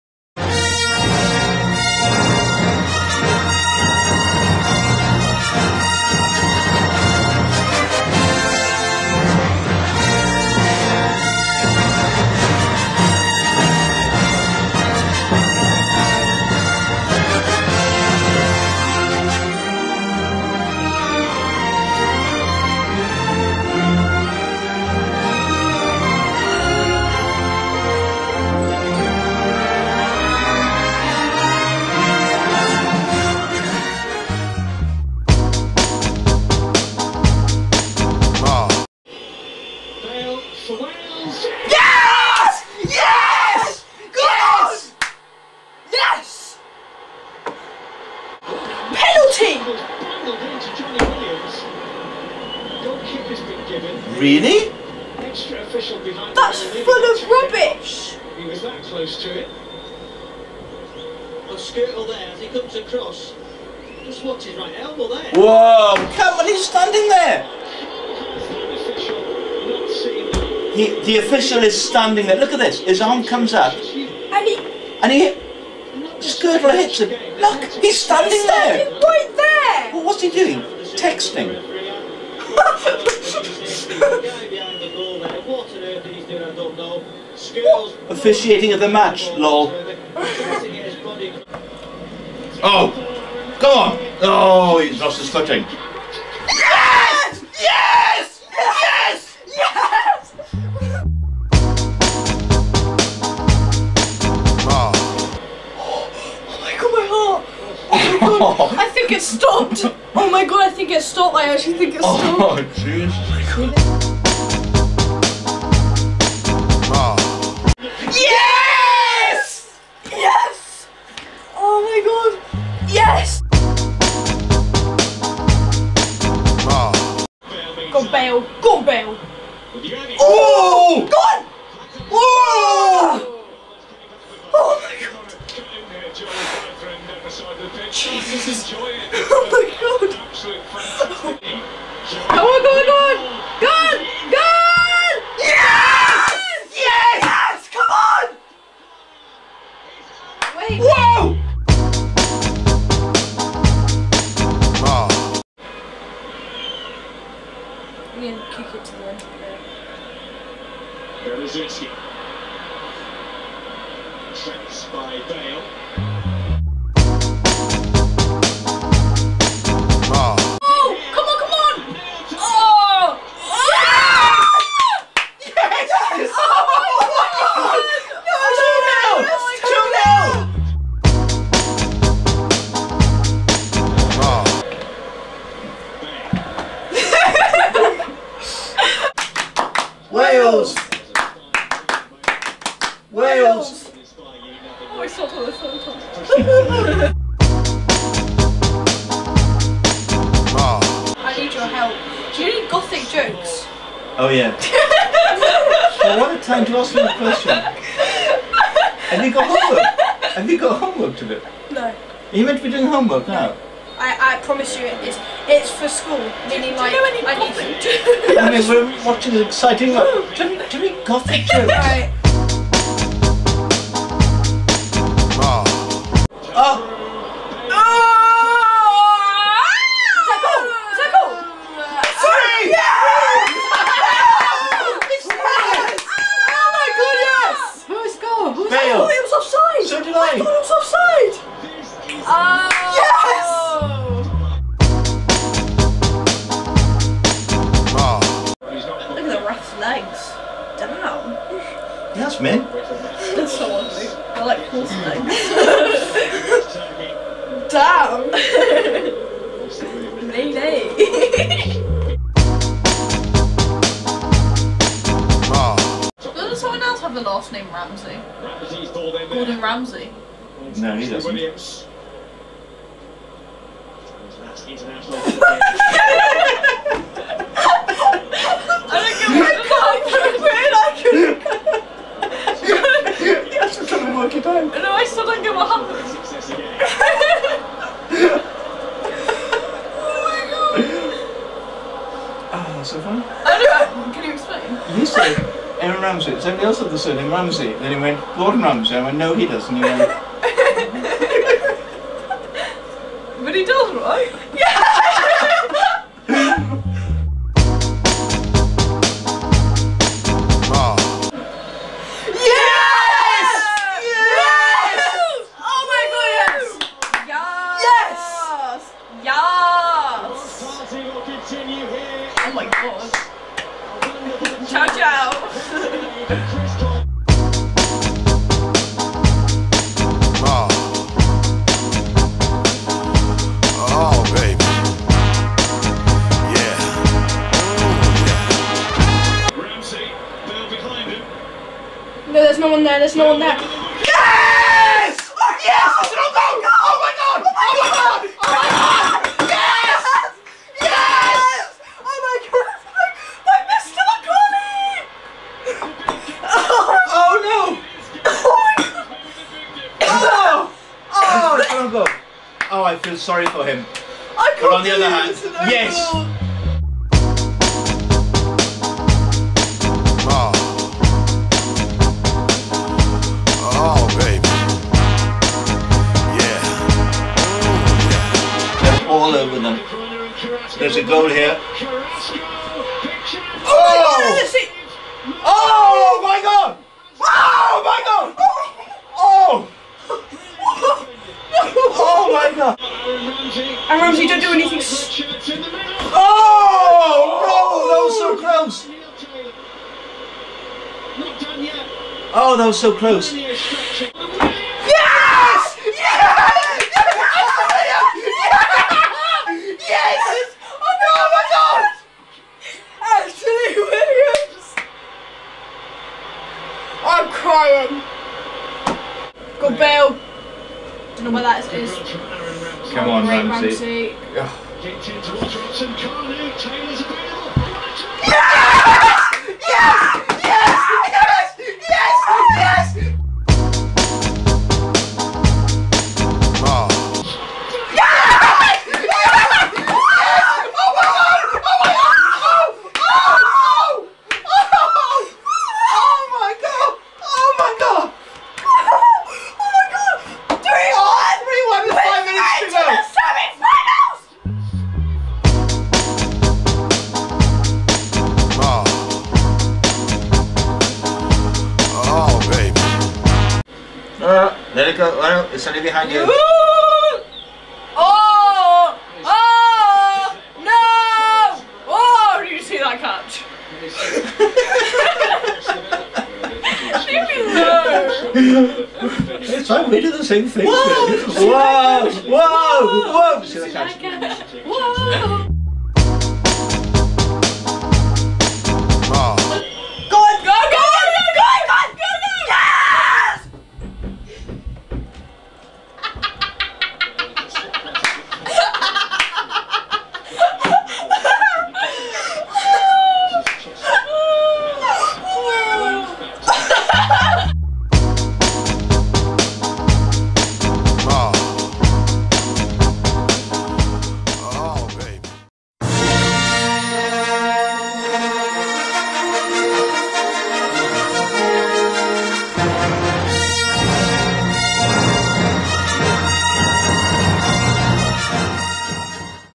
Wales Watching at Euros 2016
Comedian Bennett Arron and his children watch Wales play in their matches at Euros 2016. The nasty trick the children play on their father during the final match is worth waiting for.